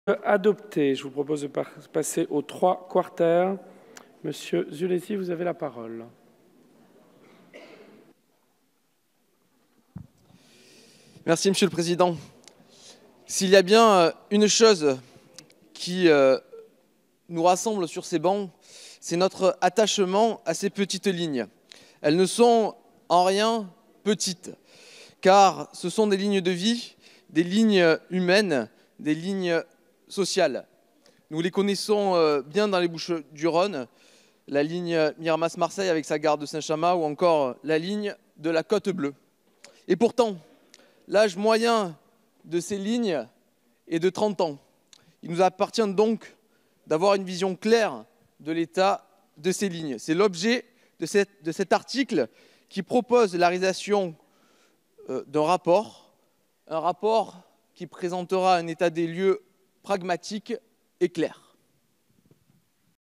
Jean-Marc Zulesi défend un audit des "petites lignes" ferroviaires
Le 11 avril 2018, à l'Assemblée nationale. Examen en séance publique du projet de loi pour un Nouveau pacte ferroviaire.